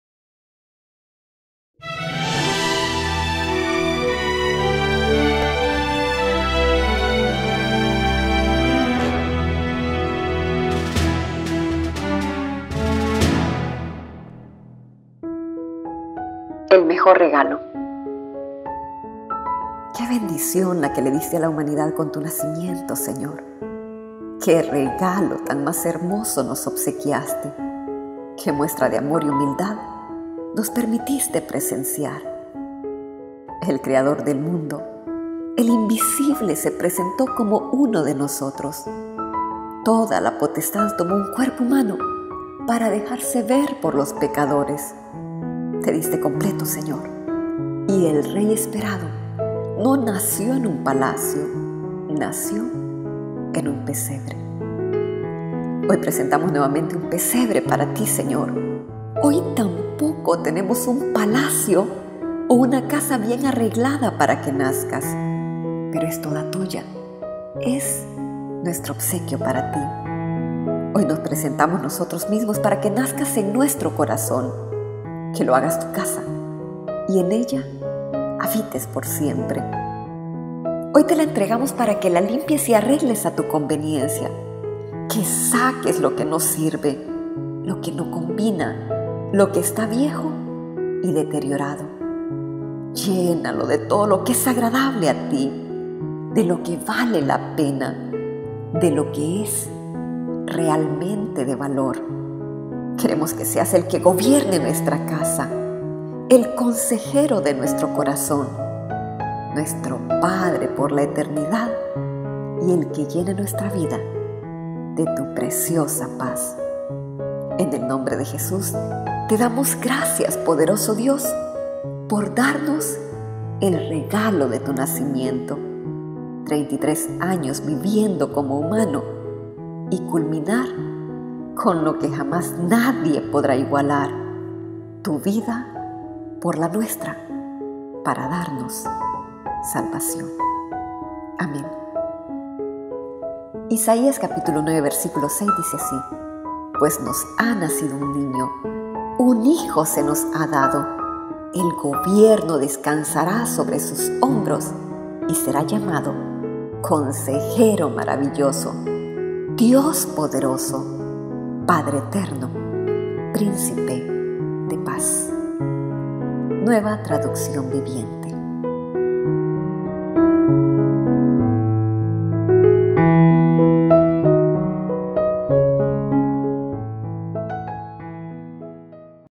ORACIONES PARA REFLEXIONAR "EL MEJOR REGALO"
*EL MEJOR REGALO*
Que bendición la que le diste a la humanidad con Tu nacimiento Señor, que regalo tan más hermoso nos obsequiaste, que muestra de Amor y humildad nos permitiste presenciar.
El Creador del mundo, el invisible se presentó como uno de nosotros; toda la potestad tomó un cuerpo humano, para dejarse ver por los pecadores.
Te diste completo Señor, y el Rey esperado, no nació en un palacio, nació en un pesebre.
Hoy presentamos nuevamente un pesebre para Ti Señor, hoy tampoco tenemos un palacio o una casa bien arreglada para que nazcas; pero es toda tuya, es nuestro obsequio para Ti; hoy nos presentamos nosotros mismos, para que nazcas en nuestro corazón, que lo hagas Tu casa y en ella habites por siempre.
Hoy te la entregamos para que la limpies y arregles a Tu conveniencia, que saques lo que no sirve, lo que no combina, lo que está viejo y deteriorado. Llénalo de TODO lo que es agradable a Ti, de lo que vale la pena, de lo que es realmente de valor.
Queremos que seas el que gobierne nuestra casa, el consejero de nuestro corazón, nuestro Padre por la eternidad y el que llene muestra vida de Tu preciosa Paz.
En el nombre de Jesús te damos gracias Poderoso Dios, por darnos el regalo de Tu nacimiento, 33 años viviendo como humano y culminar con lo que jamás nadie podrá igualar… Tu vida por la nuestra… para darnos salvación.
Amén.
Isaias 9:6 (NTV) Pues nos ha nacido un niño, un hijo se nos ha dado, el gobierno descansará sobre sus hombros, y será llamado: Consejero Maravilloso, Dios Poderoso, Padre Eterno, Príncipe de Paz.